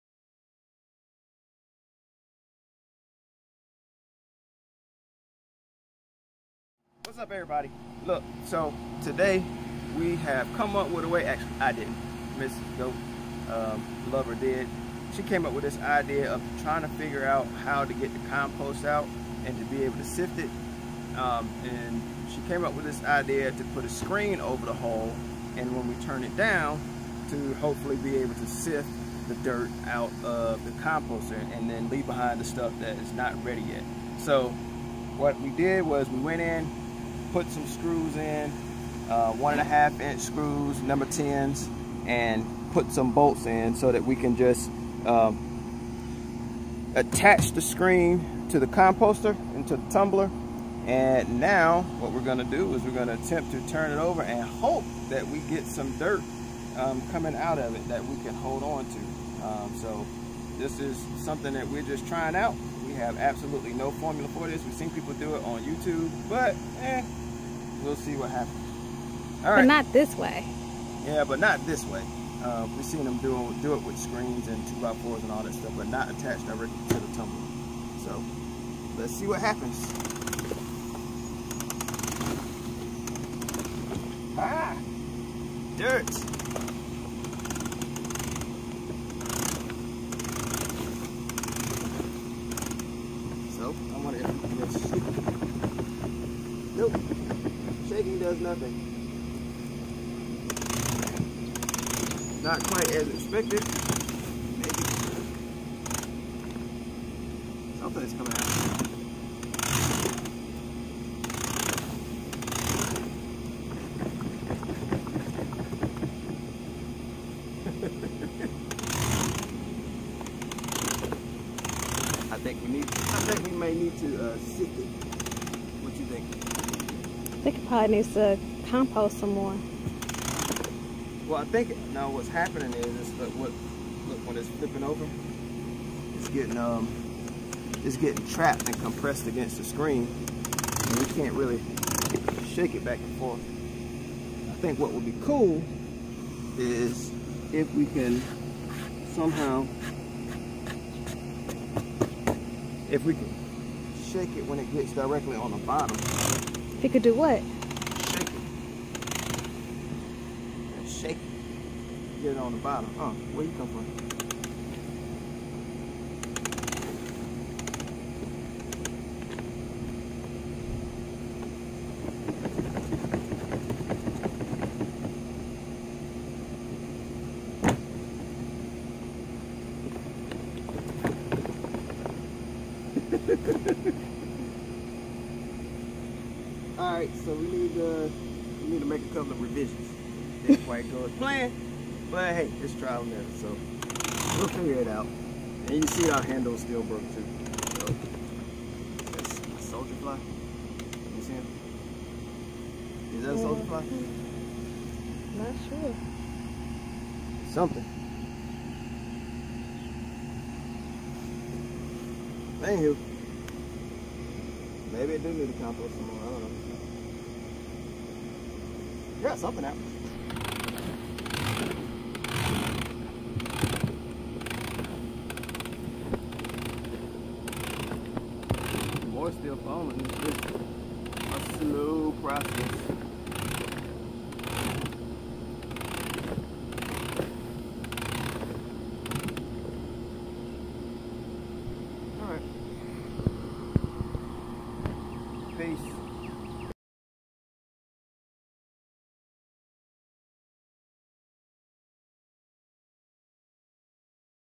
Compost Tumbler Experiment - Take 1
So, we decided to experiment with our compost tumbler.  We had been trying to figure out way to get the compost out without emptying the entire thing and we came up with an idea. This is may be the first attempt of many, but we shall see.

MATERIALS USED:
➥ 1/4 in. x 2 ft. x 5 ft. 23-Gauge Galvanized Steel Hardware Cloth
➥ #10 1" Screw with Nuts

TOOLS USED:
➥ Drill with bit
➥ Measuring tape
➥ Utility Scissors